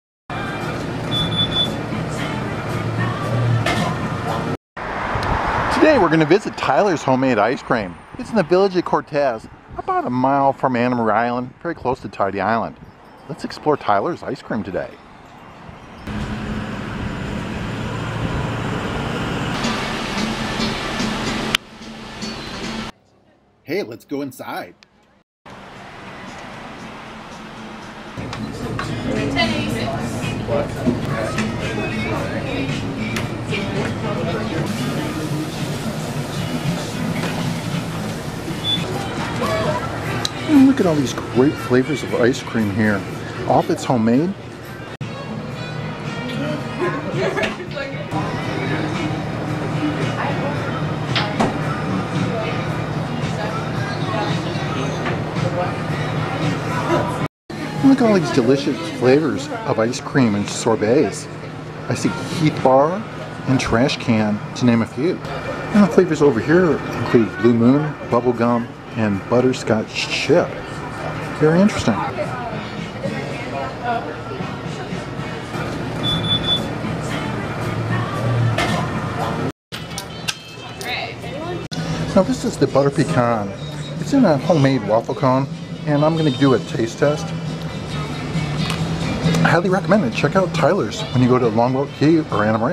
Dive into Decadent Delights at Tyler's Homemade Ice Cream in Cortez, Florida 🍦🌴
IWelcome to Tyler's Homemade Ice Cream, where you can satisfy your sweet tooth with a wide variety of delicious, handcrafted ice cream flavors in Cortez, Florida 🍦🌴. Join us as we explore this charming ice cream shop and sample their irresistible creations, made with love and the finest ingredients. From classic favorites to unique, tropical-inspired flavors, there's something for everyone at Tyler's. Don't forget to try their delightful sundaes, milkshakes, and other frozen treats 🍨. So, grab your friends and family, and let's indulge in a heavenly ice cream experience at Tyler's Homemade Ice Cream.

Timestamps:
(0:00) About Tyler's Ice Cream
(0:10) Location
(0:22) Entering Tyler's
(0:32) Ordering
(0:42) Homemade Flavors
(0:52) Hot Sundaes and Splits
(1:02) Unusual Ice Cream
(1:12) Waiting for Service
(1:22) Butter Pecan Taste Test